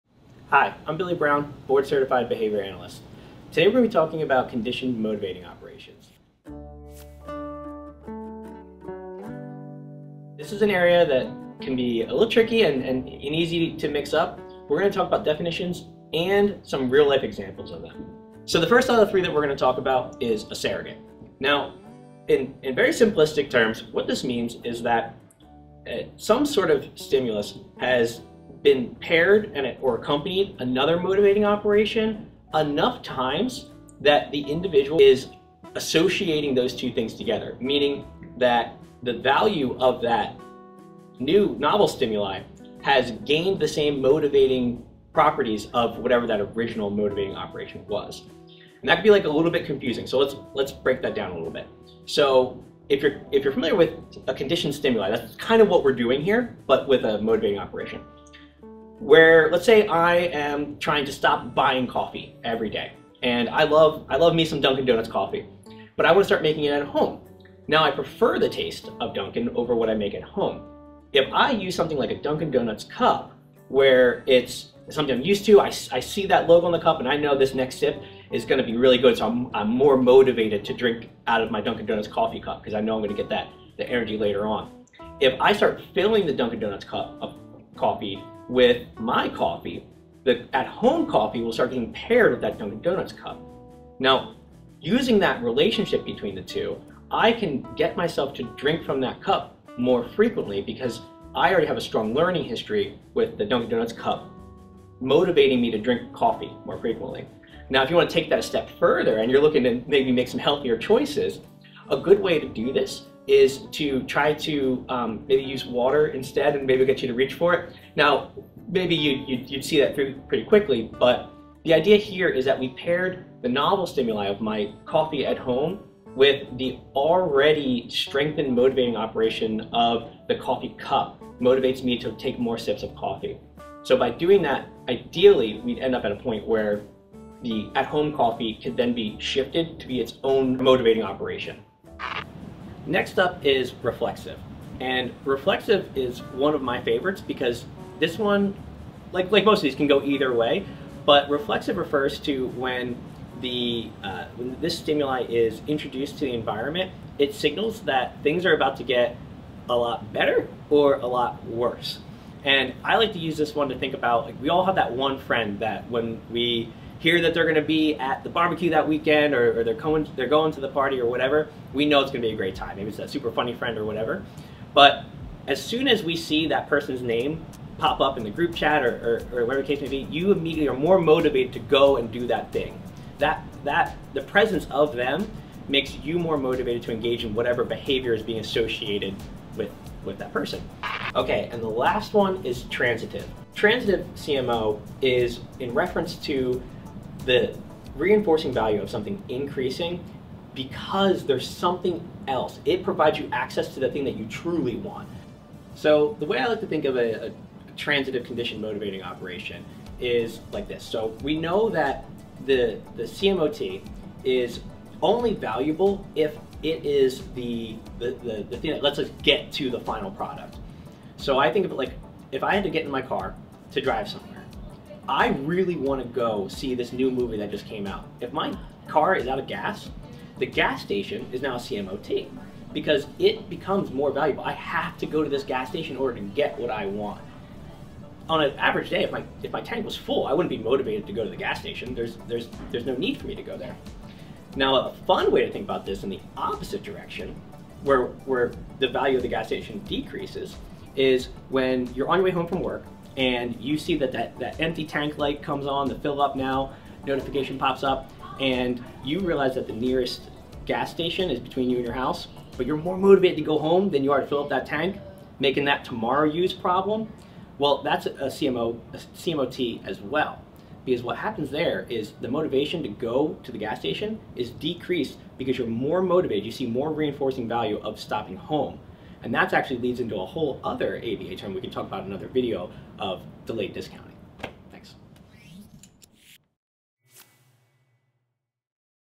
3 Conditioned Motivating Operations in Applied Behavior Analysis EXPLAINED
In this video, Board Certified Behavior Analyst, Billy Brown, provides a clear definition paired with real life, practical examples to explain 3 Conditioned Motivating Operations: Surrogate, Reflexive, and Transitive CMOs..  If you are studying for your exam and are confused about this topic, make sure you check it out and subscribe to the channel for more content!

Careers at Brett DiNovi & Associates: Now Hiring in CA, ME, NJ, NY, FL, & PA apply